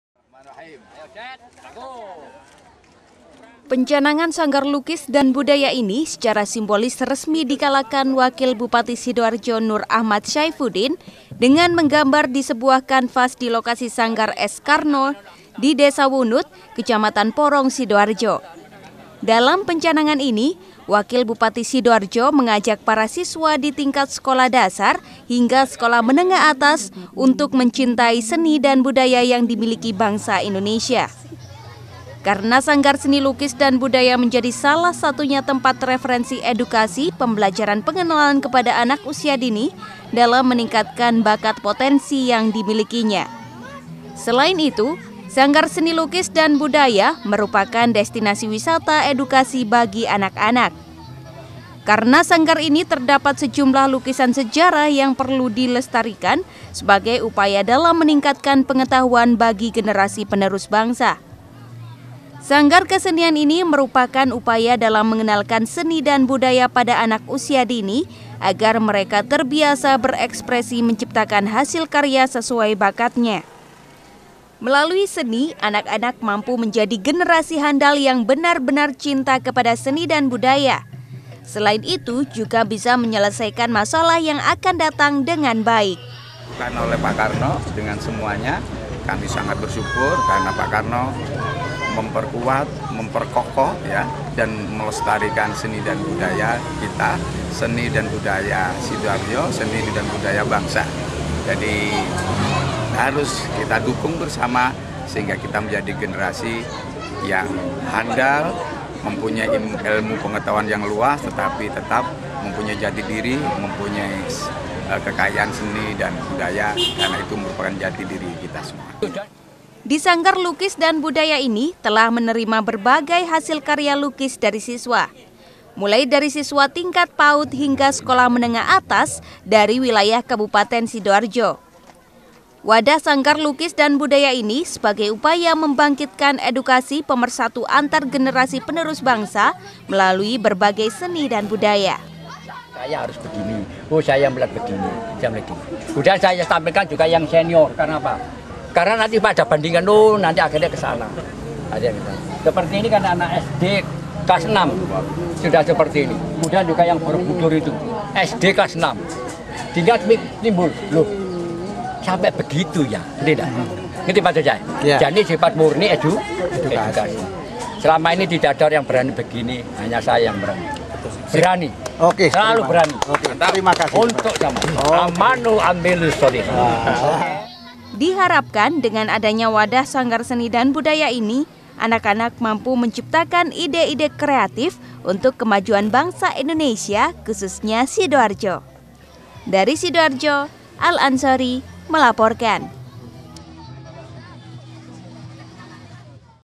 SIDOARJO - Kenalkan Anak Pada Dunia Seni
Upaya dalam mengenalkan seni dan budaya pada anak usia dini dilakukan dengan pencanangan sanggar lukis sebagai referensi edukasi bagi siswa setingkat SD hingga SMA. Di sanggar S Karno di Desa Wunut Kecamatan Porong Sidoarjo terdapat sejumlah lukisan sejarah yang perlu dilestariakan sebagai upaya dalam meningkatkan potensi bakat yang dimiliki pada anak.